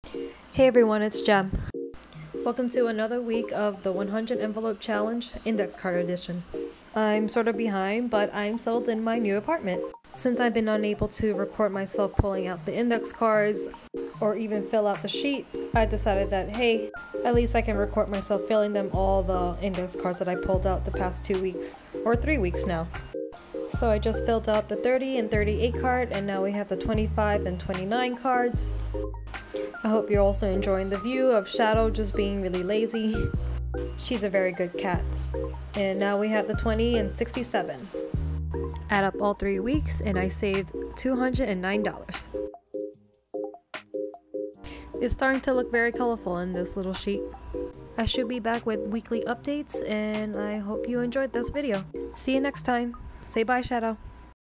100 envelope challenge (index card edition) | week 5 + 6 + 7
week 5 + 6 + 7 of my 100 envelope challenge 💸

i'm super behind, but i'm catching up now :)

i update my progress every friday!